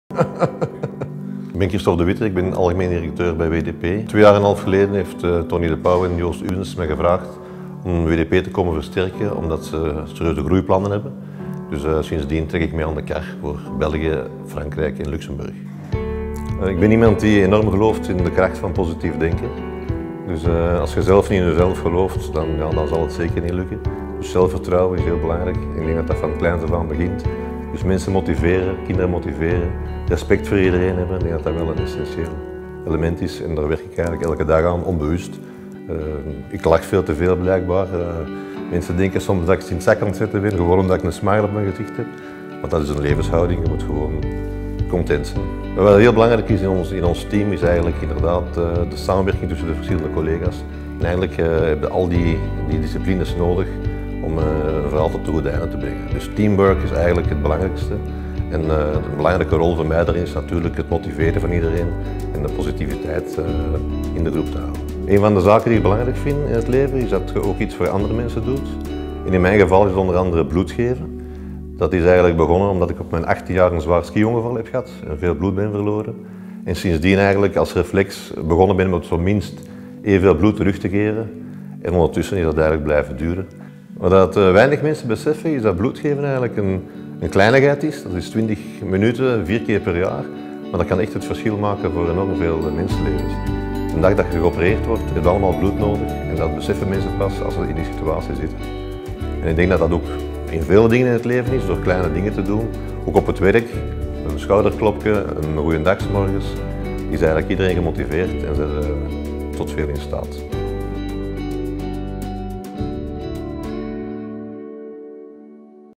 Kristof De Witte - Country Manager
Kristof firmly believes in positive thinking. We followed him when he went to Grimbergen to donate blood at the Red Cross. On the way we stopped at the well known Abbey of Grimbergen.